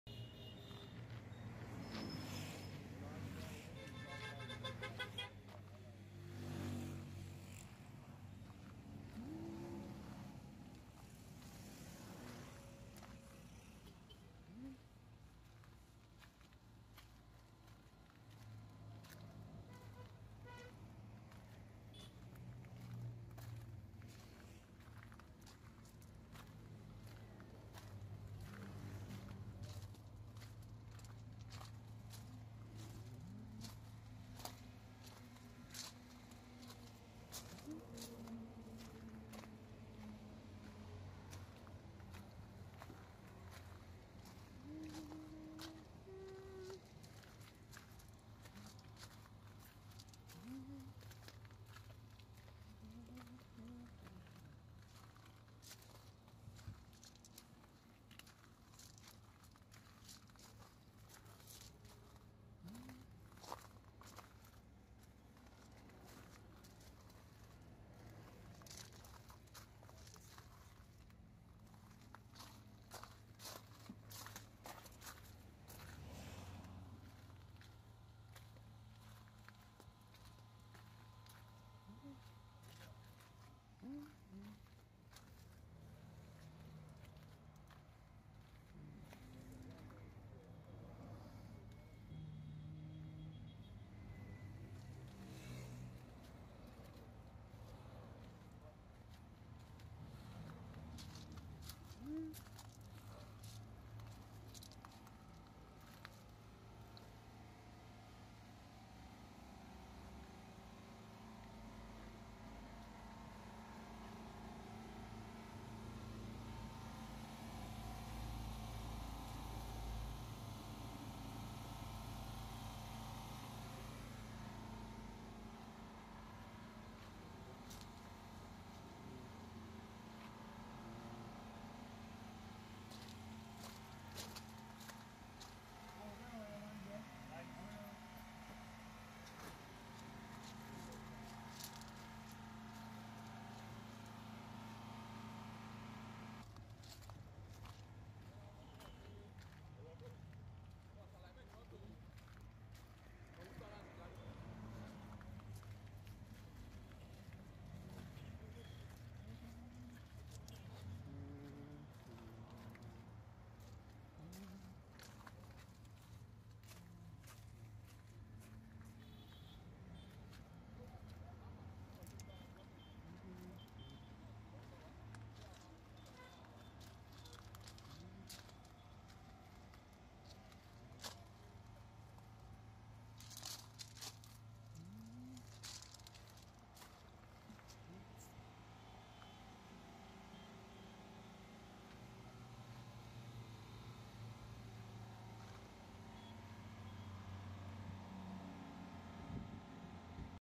Affordable foreign used cars in Ibadan | Monsaz Auto Company
Pls check out this lovely foreign used cars at Monsaz Auto Company, don't forget to call them to negotiate anyone of your choice. Thank you